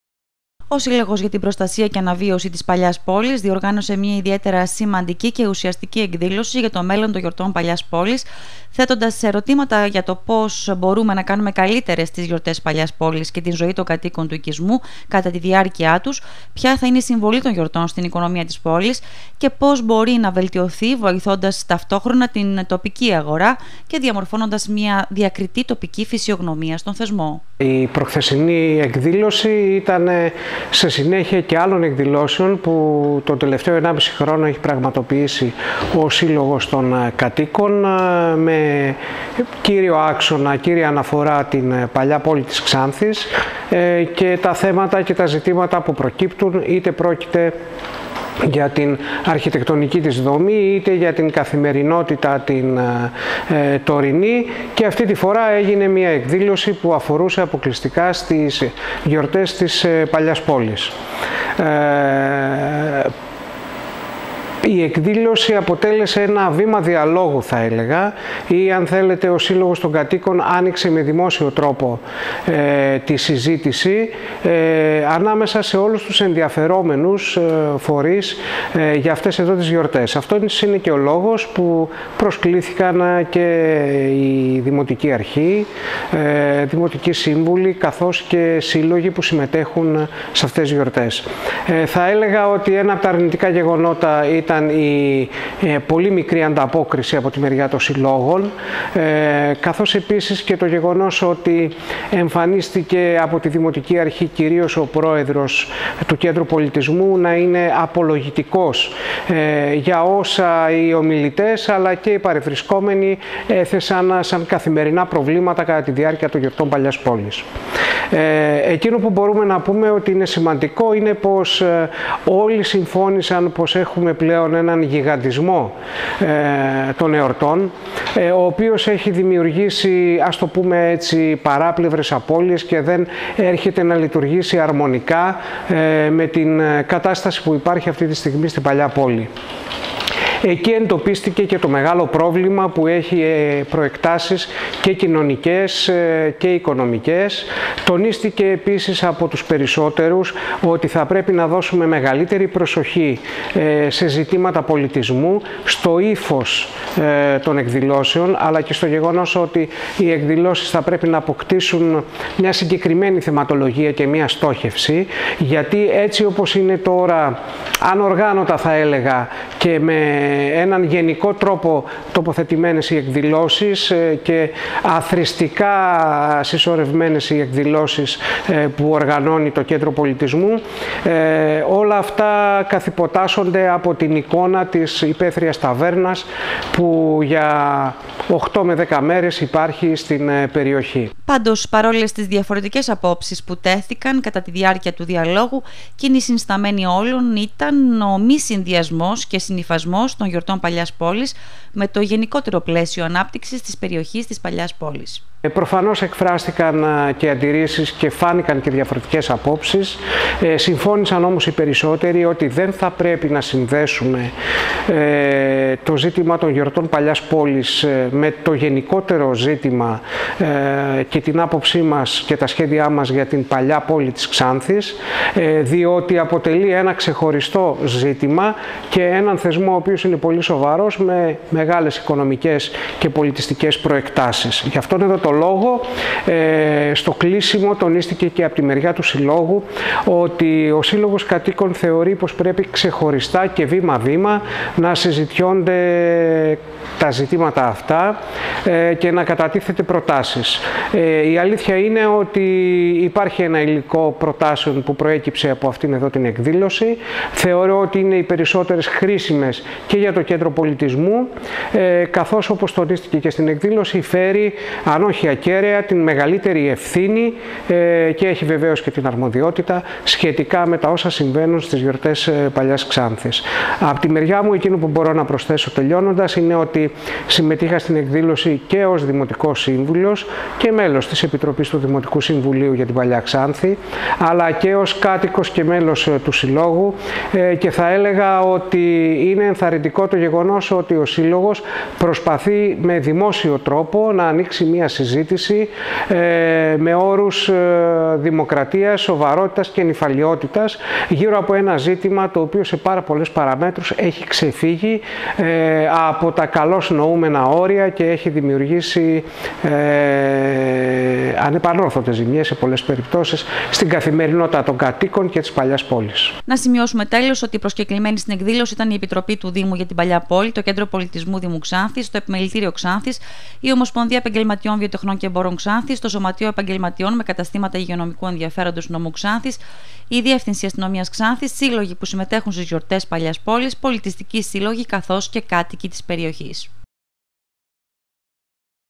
ΕΡΩΤΗΜΑΤΑ ΓΙΑ ΤΙΣ ΓΙΟΡΤΕΣ ΠΑΛΙΑΣ ΠΟΛΗΣ ΜΕ ΑΙΧΜΗ ΤΟΝ ΣΕΒΑΣΜΟ ΣΤΟΝ ΟΙΚΙΣΜΟ ΚΑΙ ΣΤΟΥΣ ΚΑΤΟΙΚΟΥΣ ΤΟΥ
ΕΡΩΤΗΜΑΤΑ ΓΙΑ ΤΙΣ ΓΙΟΡΤΕΣ ΠΑΛΙΑΣ ΠΟΛΗΣ ΜΕ ΑΙΧΜΗ  ΤΟΝ ΣΕΒΑΣΜΟ ΣΤΟΝ ΟΙΚΙΣΜΟ ΚΑΙ ΣΤΟΥΣ ΚΑΤΟΙΚΟΥΣ ΤΟΥ 1-7-2018